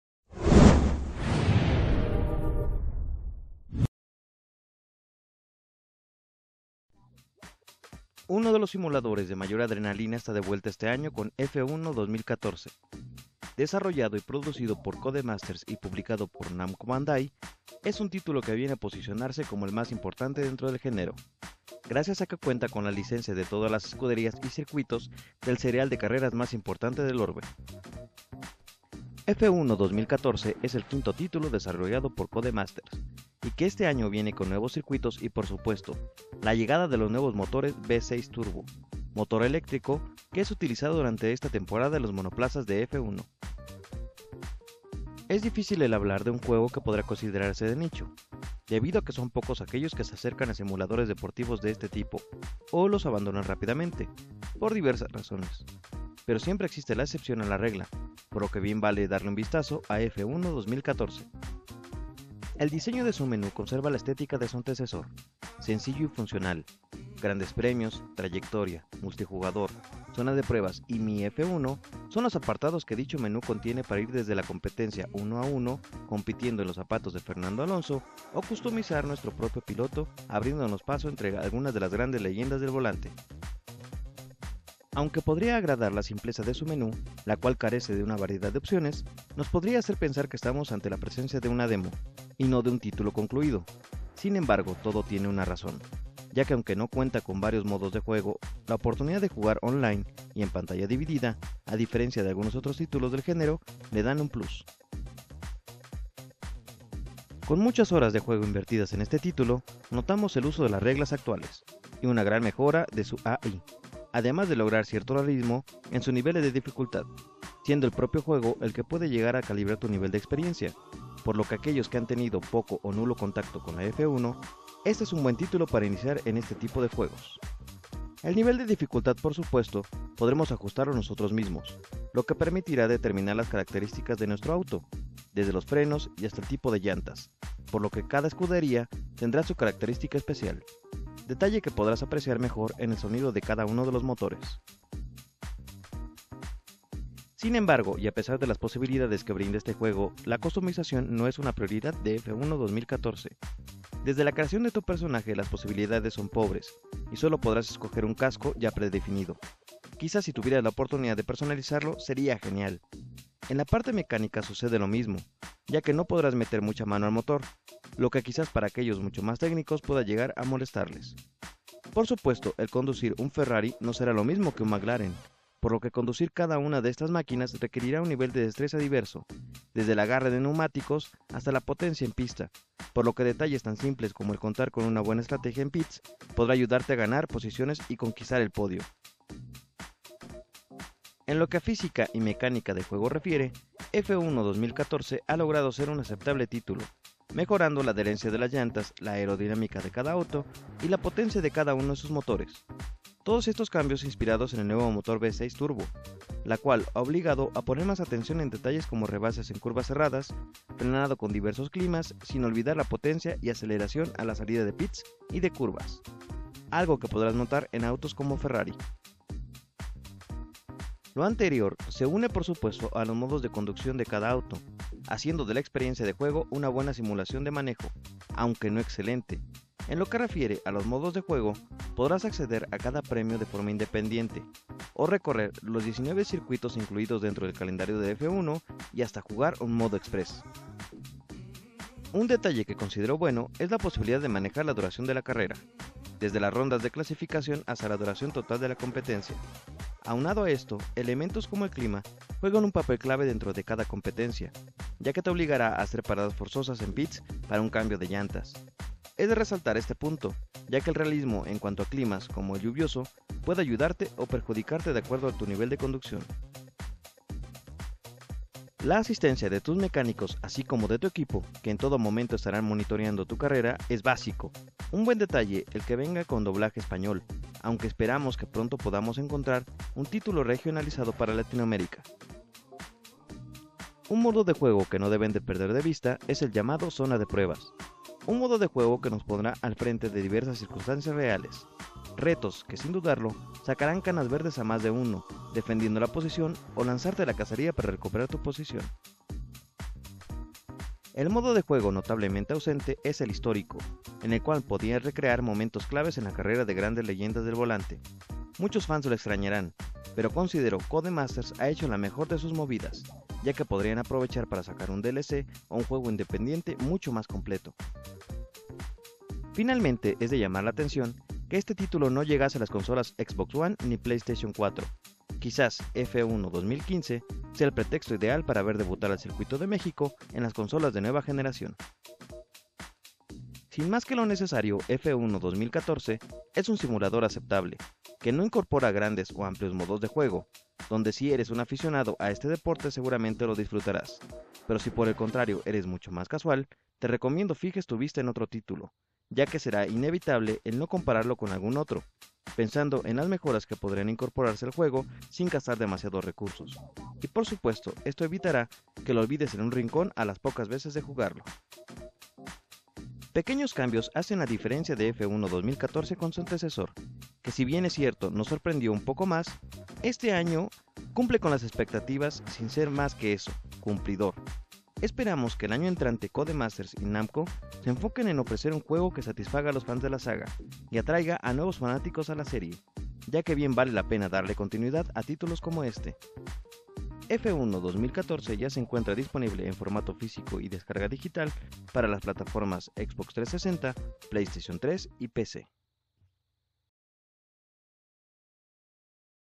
Videoreseña: F1 2014
CODEMASTERS y BANDAI NAMCO traen el nuevo simulador de automovilismo F1 2014, con nuevas características y algunos cambios que los fans han de considerar para adquirirlo.
¿Cumplirá F1 2014, la exigencia de los fans? no te pierdas nuestra vídeo reseña y no olvides visitar (noticias, vídeos, reviews, y muchos más)